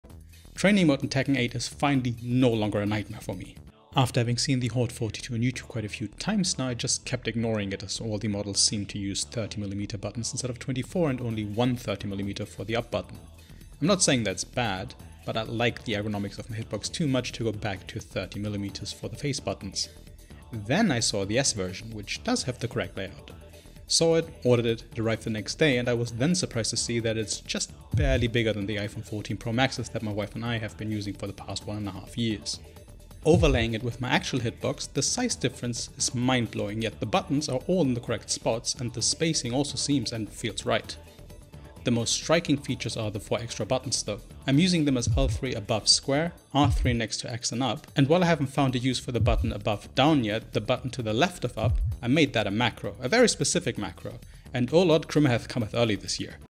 Switching to Haute42 S16? The Ultimate Leverless Experience? Hit Box?
Discover the game-changing Haute42 S16, a leverless arcade controller that cost me 68.39€ and redefines my quality of life playing Tekken 8. After years of loyalty to the traditional Hit Box setup, I explore the Haute42 S16 version, with its Hit Box conform leverless layout and compact design that is just barely bigger than an iPhone 14 Pro Max. That's a significant size difference, when compared to the Hit Box, but we are still looking at correct button placement and the optimal spacing that OG Hit Box offers. I need some more long term use to really get an idea if the Haute42 S16 is worth its price tag, but at less than 70€ and how good it feels already... I hope it can make it!